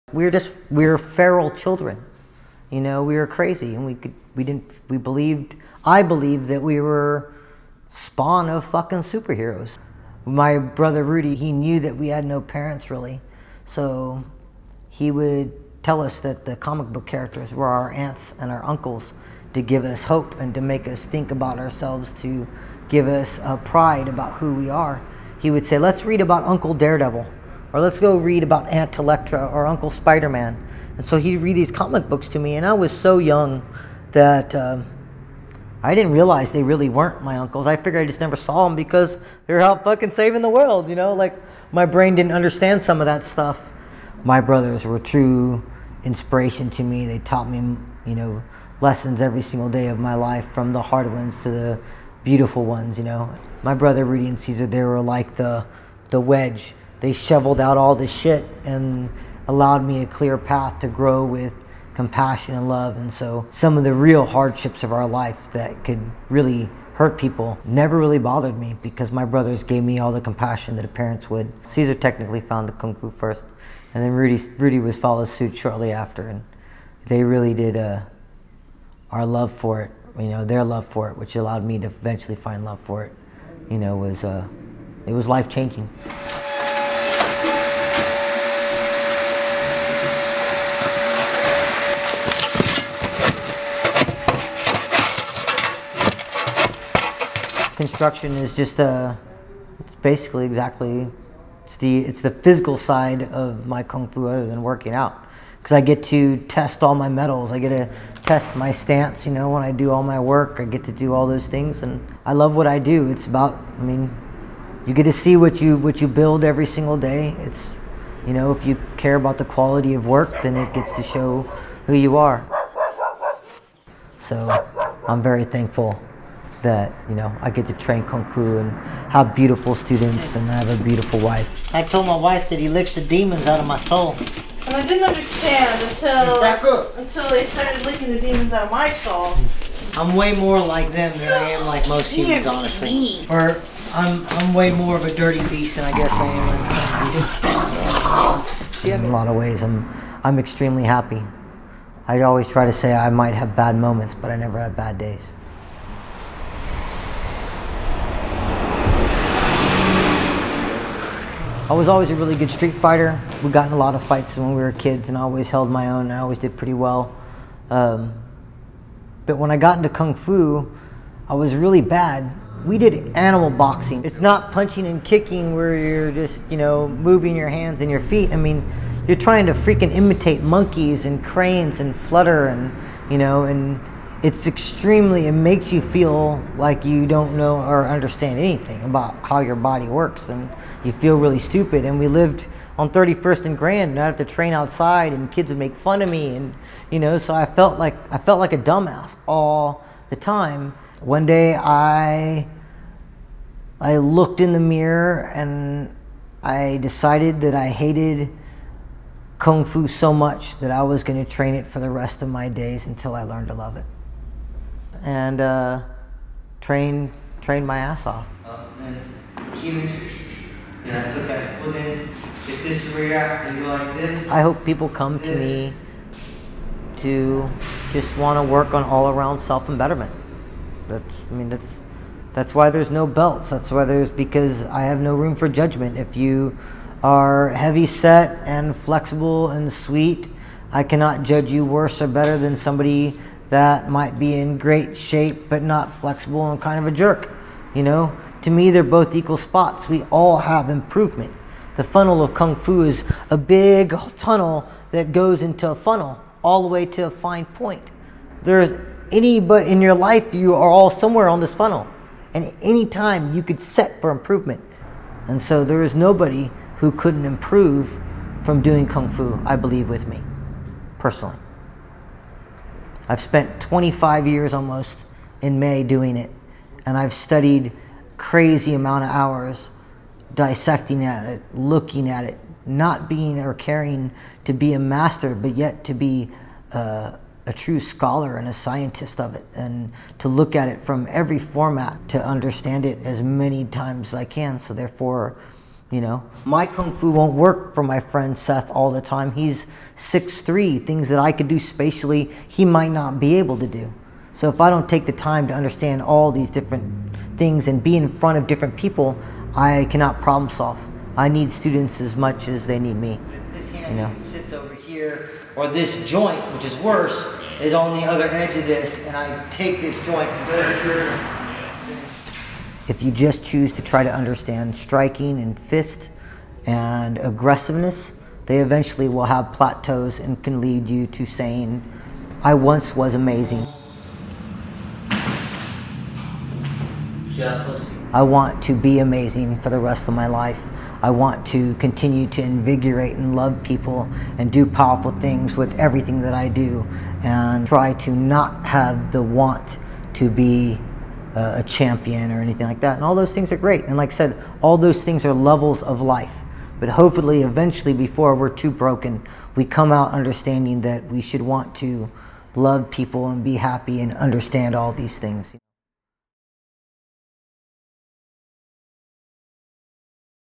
Mikey | A Kansas City Story
This is a Kansas City Story about Mikey.  Mikey has been training in Kung Fu since he was a young man. This is about what that means to him.

This film was photographed by Brandon Parigo
Produced by Nicole and Brandon Parigo
Family photographs provided by Mikey